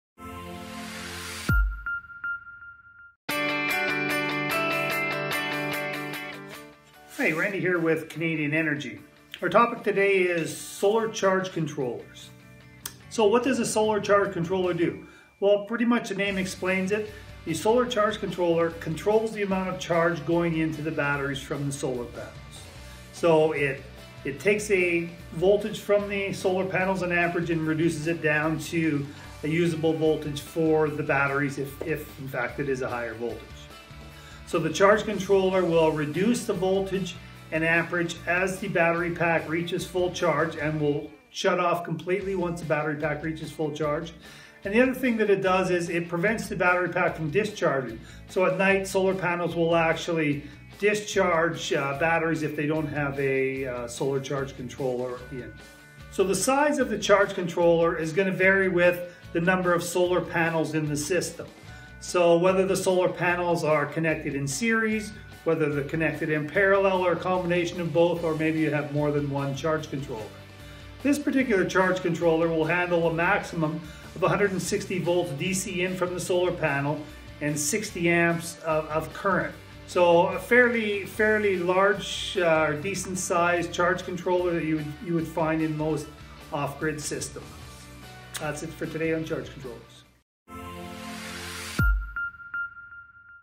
Canadian Energy™ - Living Off-Grid 101 - Solar Charge Controllers (7/12)
What is a solar charge controller? What does it do for my off-grid system? Why do I need one? All these answers and more in this video about solar charge controllers as a critical component to off-grid solar systems.